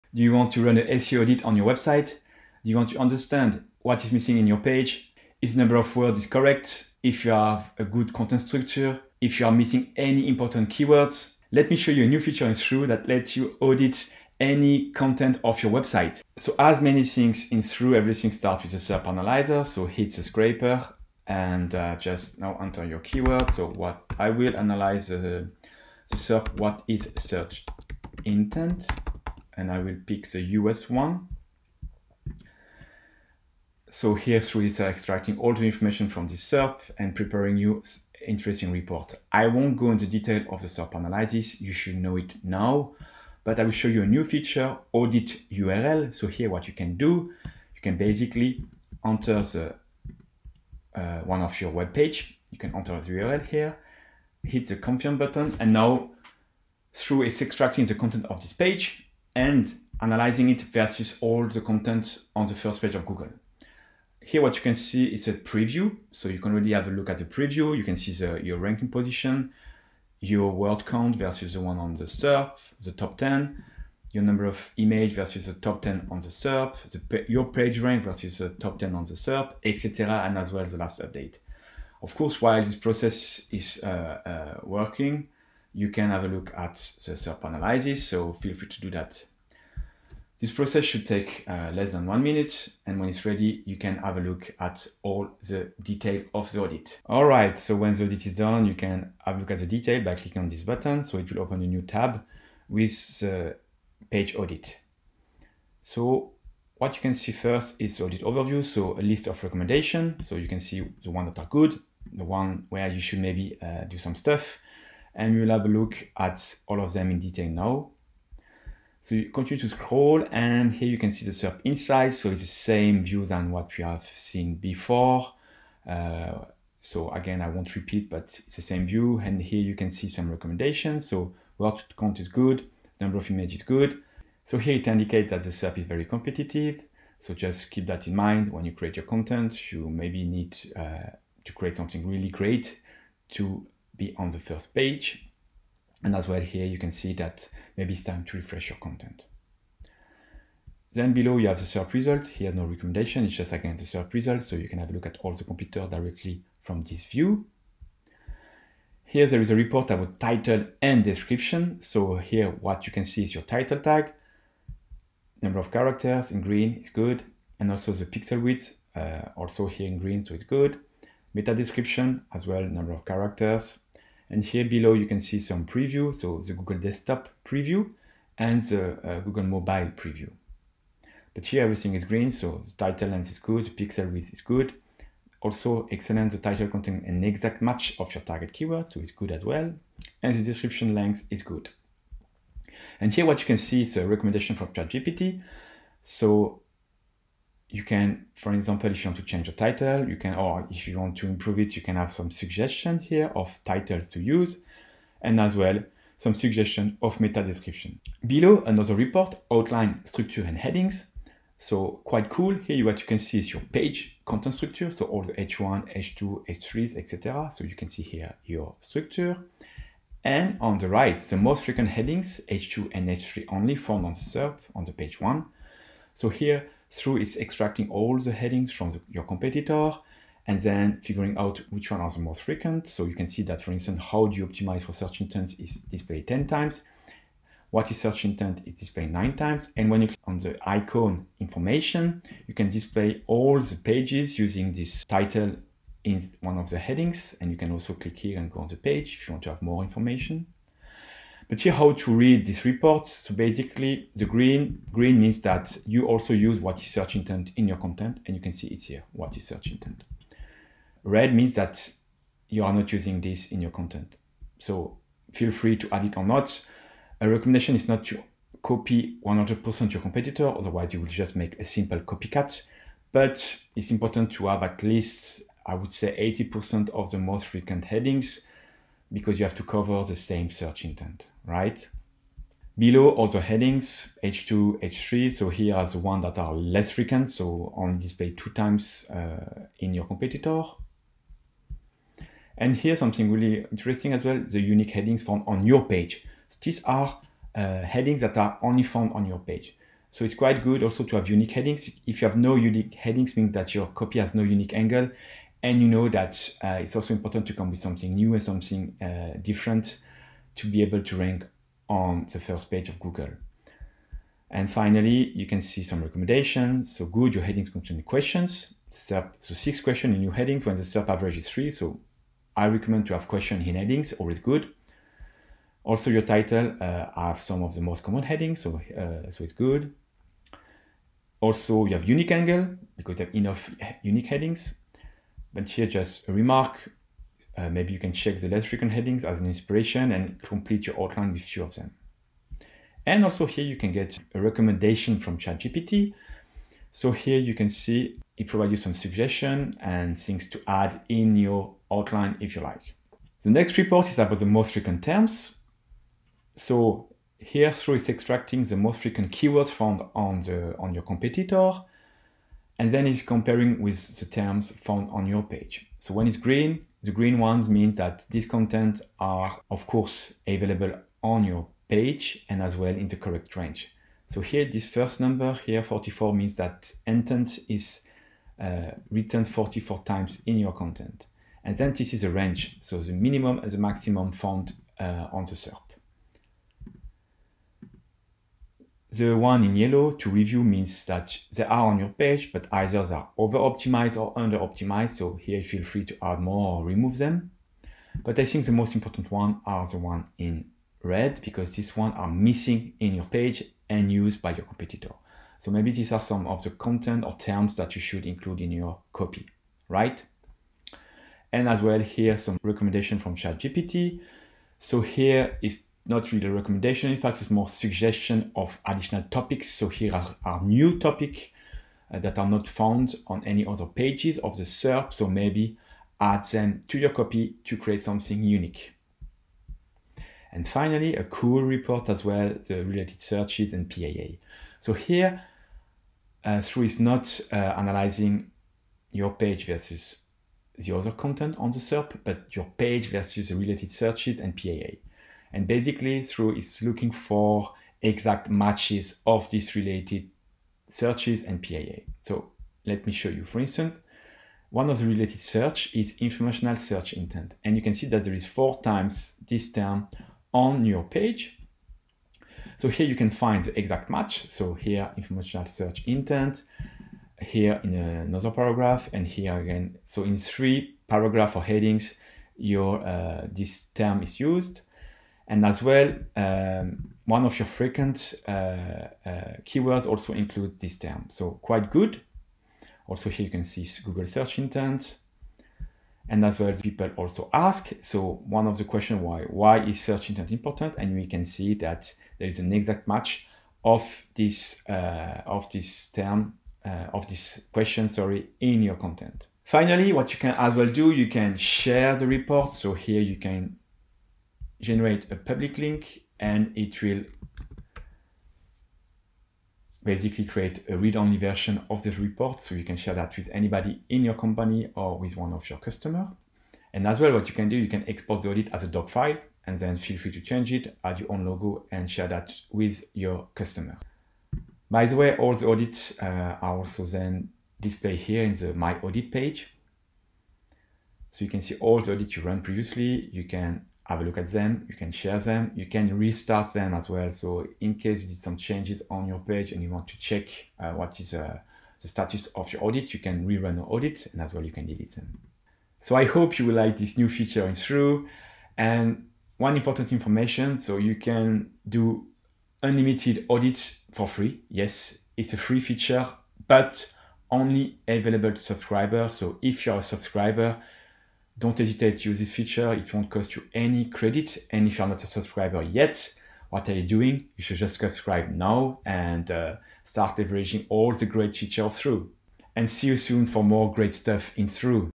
Automated SEO Page Audit (Analyze your Page vs. Google SERP)
Discover the power of SEO Page Audit, the ultimate tool for optimizing your content and achieving higher search engine rankings.

In this feature showcase video, we dive deep into how thruuu's SEO Page Audit empowers you to compare your page against competitors, receive personalized recommendations, analyze missing keywords, optimize heading structure, and more.

Learn how to leverage real-time data and AI-powered insights to elevate your SEO strategy and stay ahead of the competition. Don't miss out on this game-changing feature – watch now and take your SEO efforts to new heights!